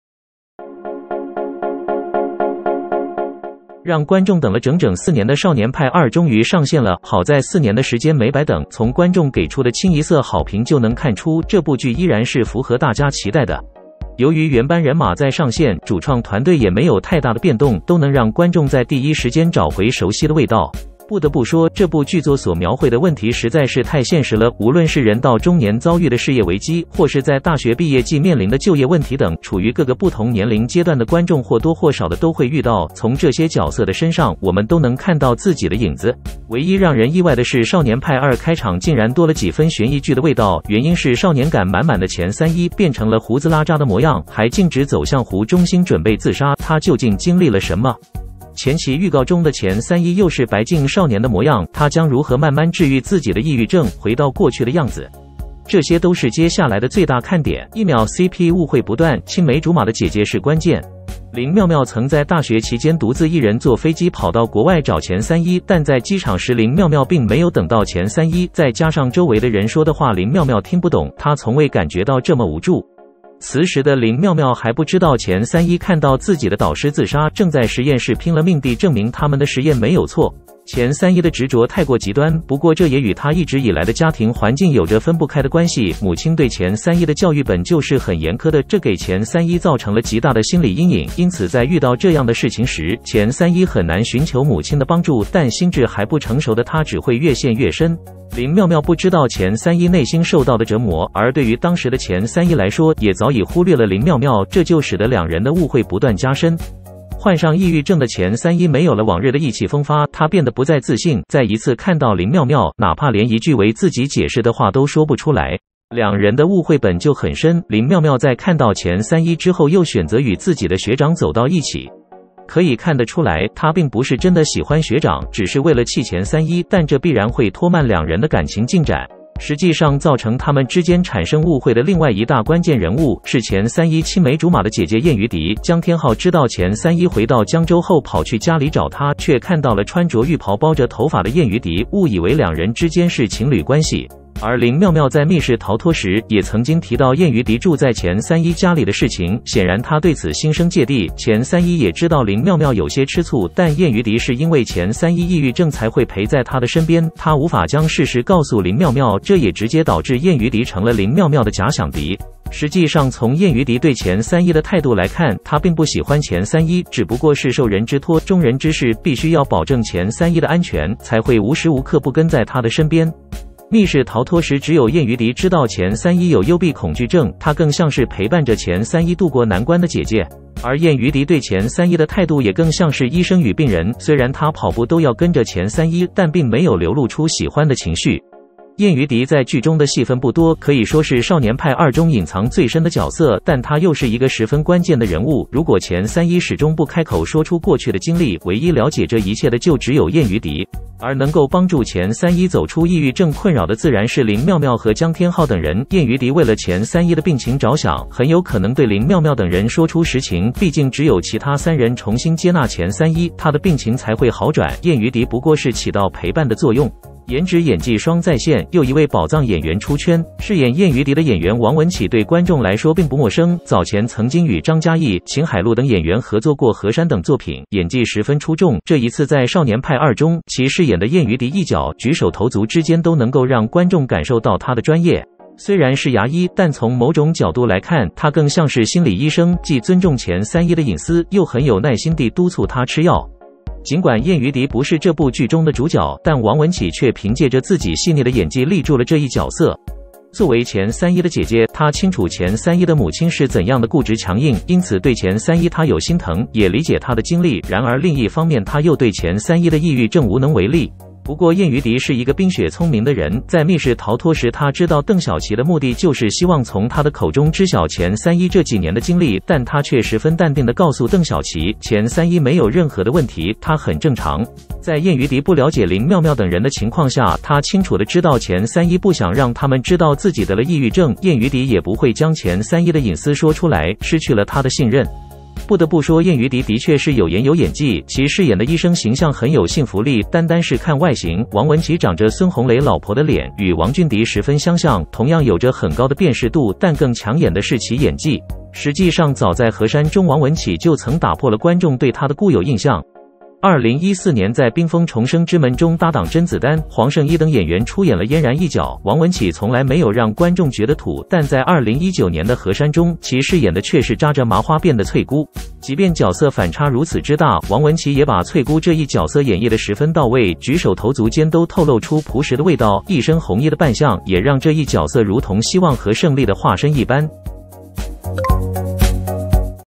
《少年派2》隱藏最深的角色，長著孫紅雷老婆的臉，演技炸裂 | 一聞演藝
讓觀眾等了整整四年的《少年派2》終於上線了，好在四年的時間沒白等，從觀眾給出的清一色好評就能看出，這部劇依然是符合大家期待的。由於原班人馬在上線，主創團隊也沒有太大的變動，都能讓觀眾在第一時間找回熟悉的味道。